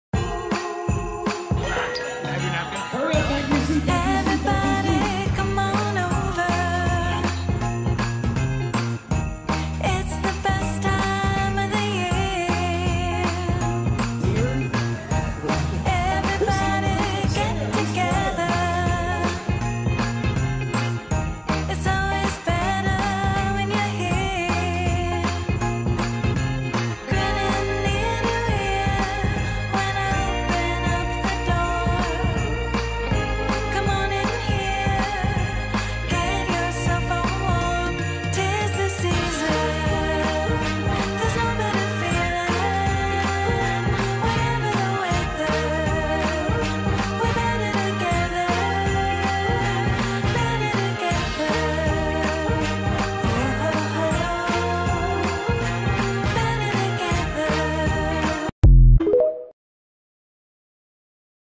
2023 Team Holiday Party was a great time last night 🎄🎁. Surrounded by such positive and fun people!
2023 Team Holiday Party was a great time last night 🎄✨️🎁. Nothing better to be surrounded by such positive and fun people!